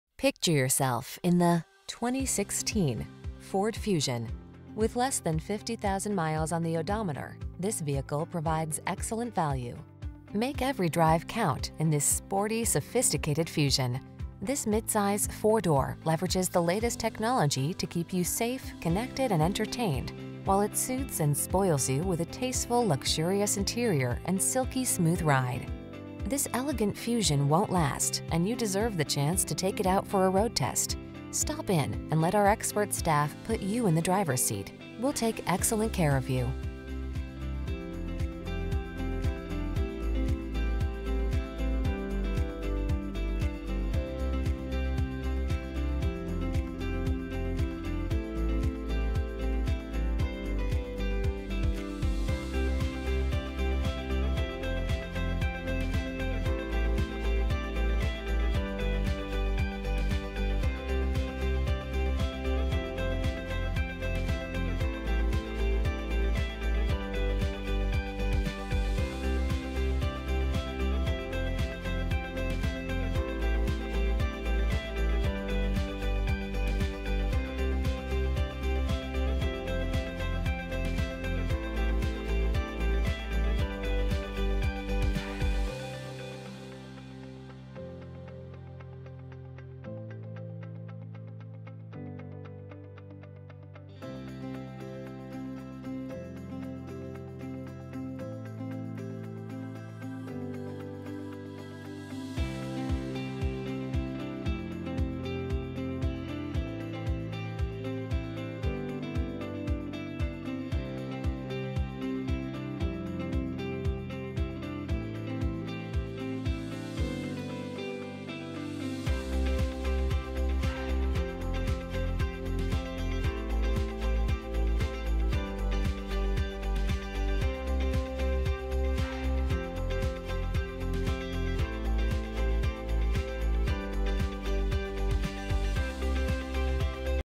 2016 Ford Fusion Redding, Eureka, Red Bluff, Northern Corning California, CA 131242
BRONZE FIRE Used 2016 Ford Fusion available in Redding, California at Crown Motors Redding. Servicing the Eureka, Red Bluff, Northern California, Corning, CA area.
2016 Ford Fusion S - Stock#: 131242 - VIN#: 3FA6P0G71GR357423